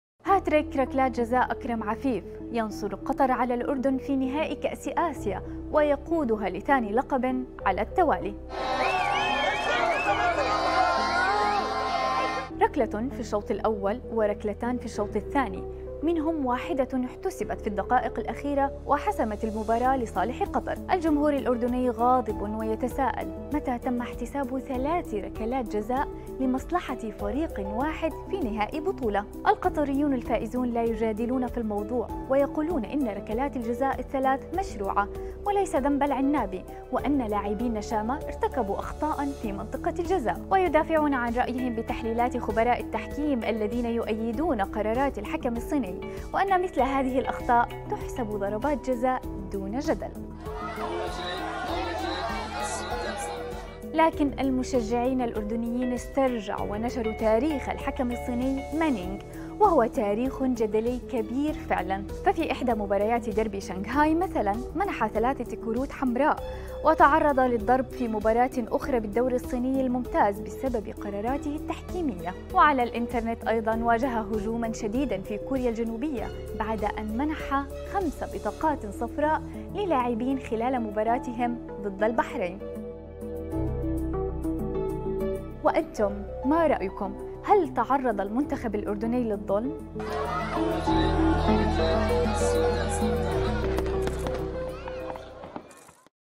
"العنابي" يفوز على الأردن بثلاثية في نهائي آسيا وسط جدل تحكيمي
الأردن ضحية أخطاء التحكيم أمام قطر بـ كأس_آسيا؟.. تاريخ الحكم الصيني"ما نينغ" يثير الشكوك
لمعرفة أحدث المستجدات.. عبر البث المباشر من هنا ⤵
وبإمكانكم متابعة العربية على منصات التواصل